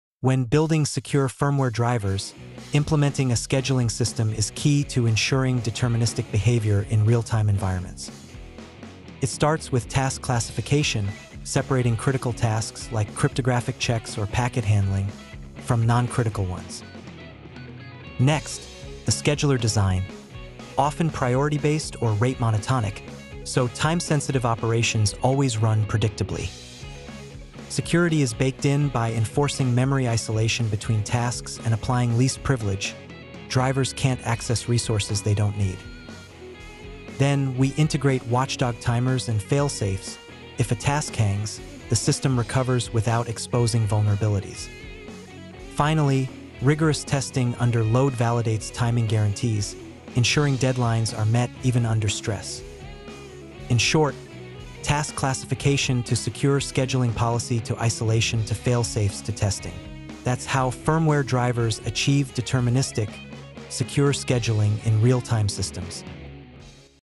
Secure Firmware Driver Development
In secure firmware drivers, scheduling isn’t just about timing, it’s about deterministic, predictable behavior under real-time constraints.

From classifying tasks, enforcing isolation, and using priority-based scheduling, to watchdog fail-safes and load testing, each step keeps systems reliable and secure.

Determinism = Security in real-time systems.